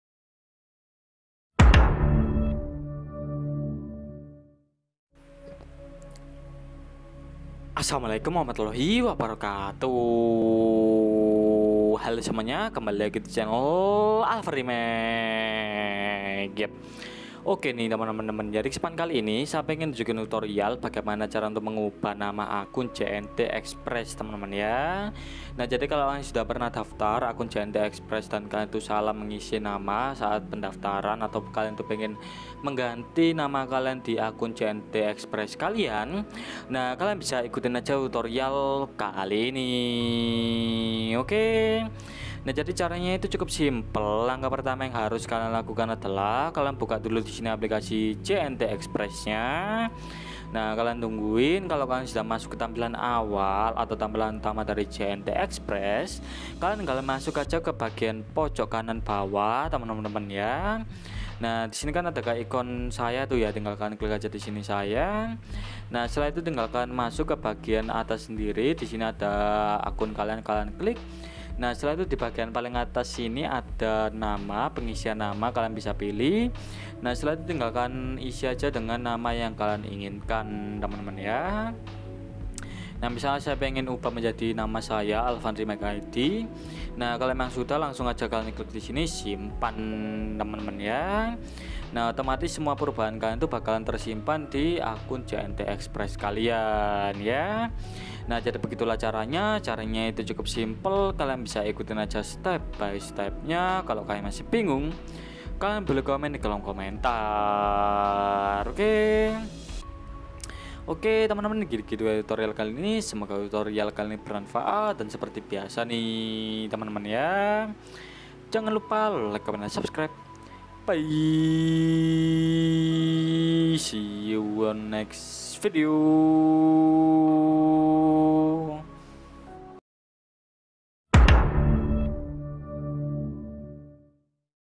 CARA MENGUBAH NAMA AKUN JNT/J&T EXPRESS
Assalamu’alaikum Wr.WB
Dalam video tutorial ini, kita akan membahas
CARA MENGUBAH NAMA AKUN JNT/JN&T EXPRESS